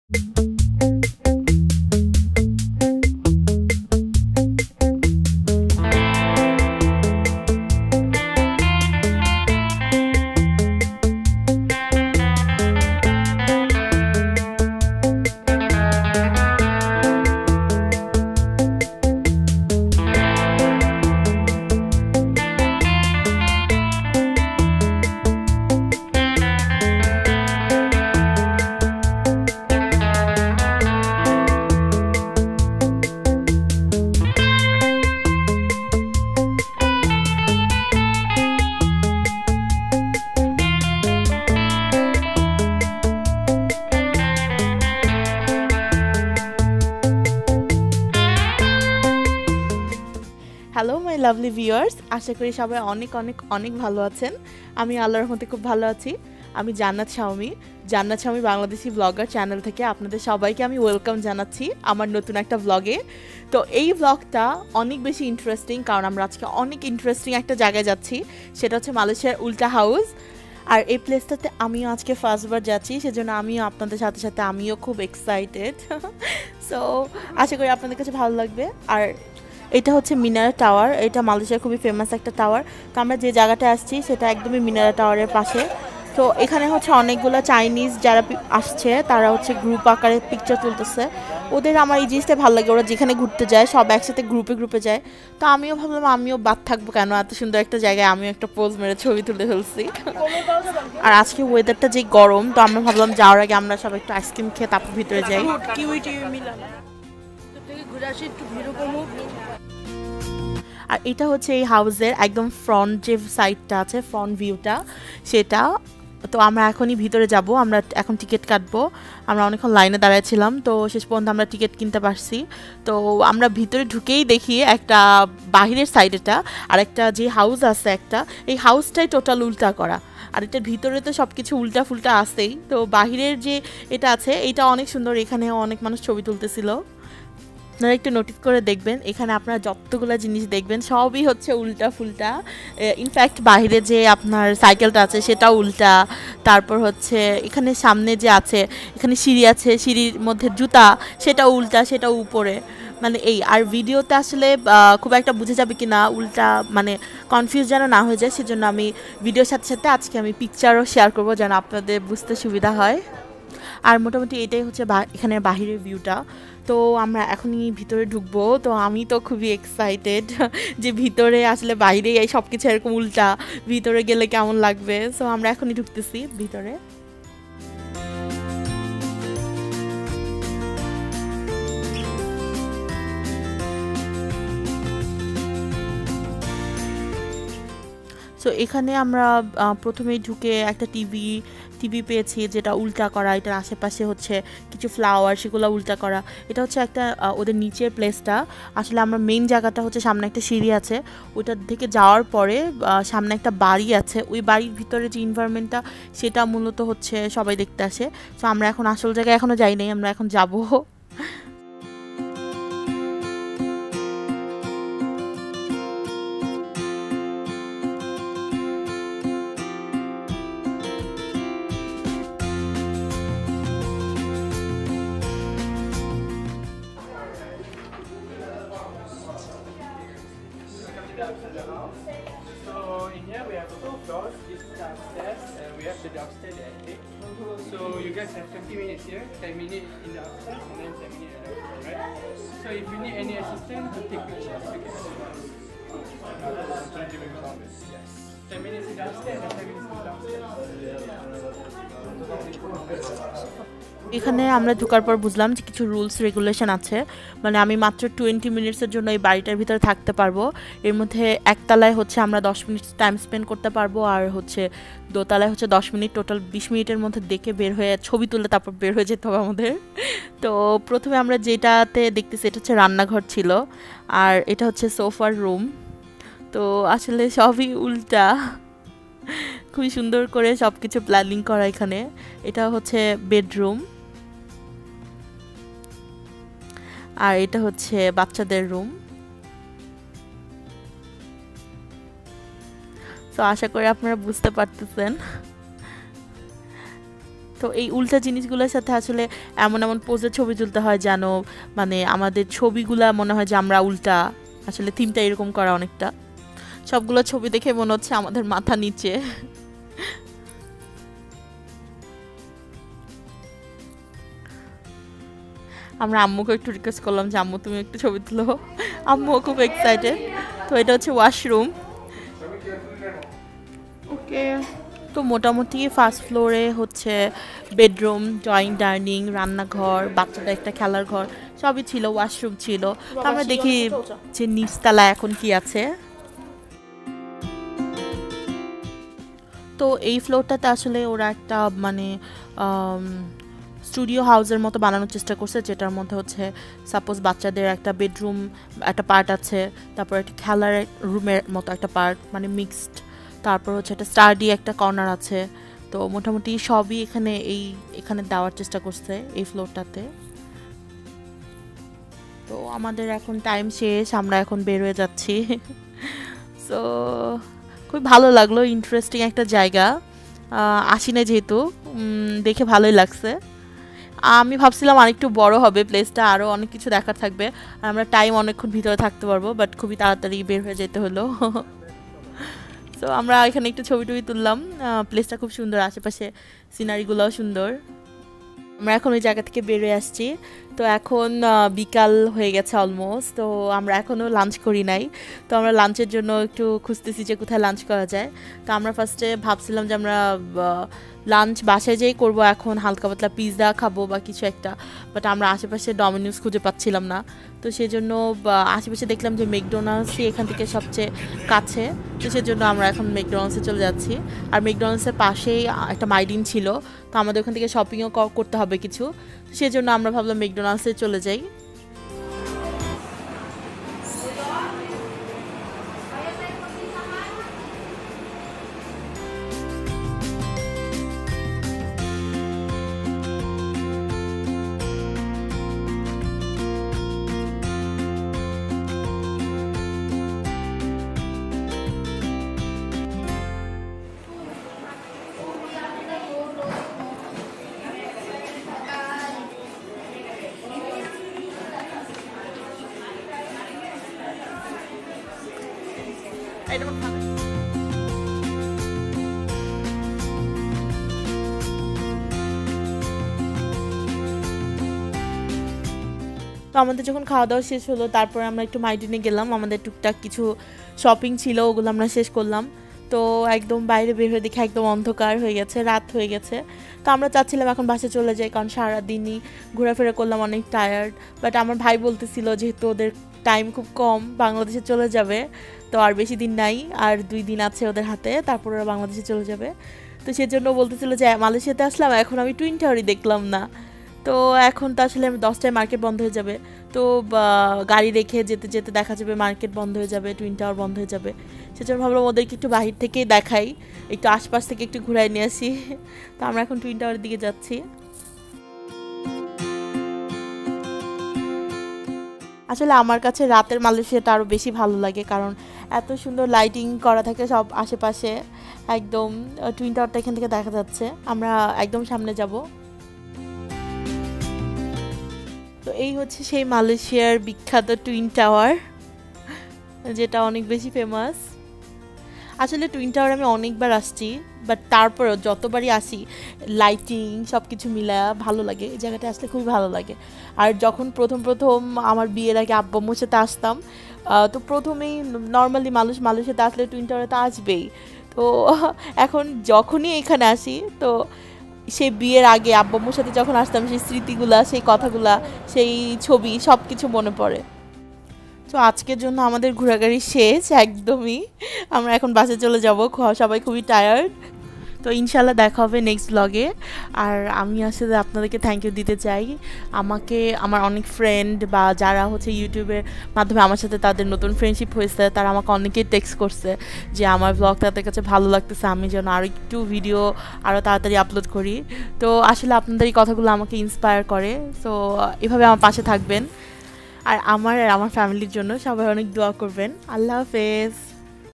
Malaysia UPSIDE DOWN HOUSE || travel vlogs || মালেশিয়ার উল্টো বাড়ি দেখতে কেমন?!!!
Today we are going to KL Upside Down House which is situated in kuala lumpur,Malaysia.This place is very interesting for every generations people specially children will love & enjoy their upside down house,I hope you guys like the video.

My facebook page limk